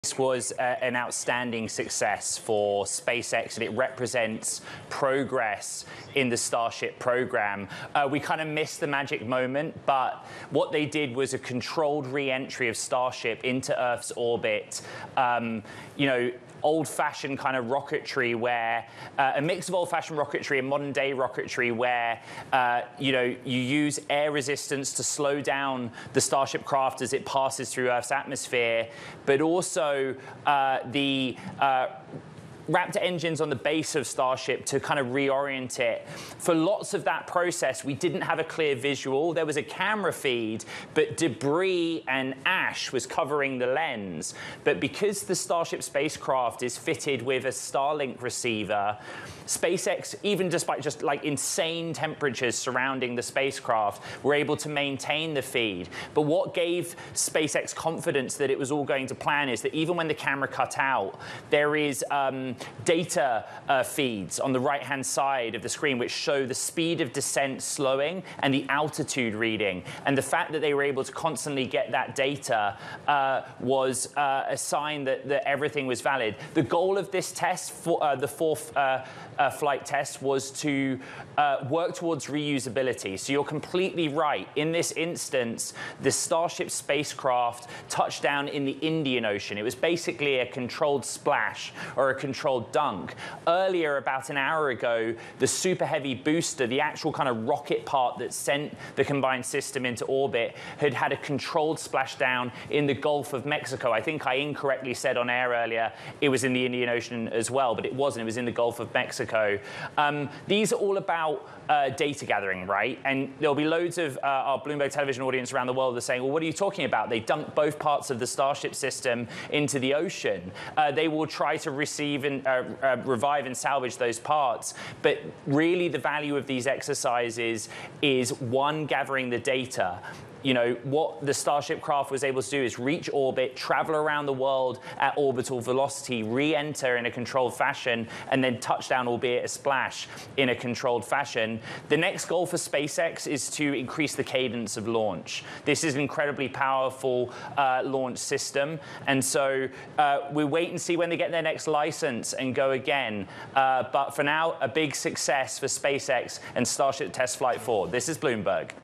SpaceX Starship Lands in Indian Ocean
The SpaceX Starship blasted off from Texas to space then came back to Earth and landed in the Indian Ocean. This was the fourth test flight for Elon Musk's massive rocket. Bloomberg's Ed Ludlow reports.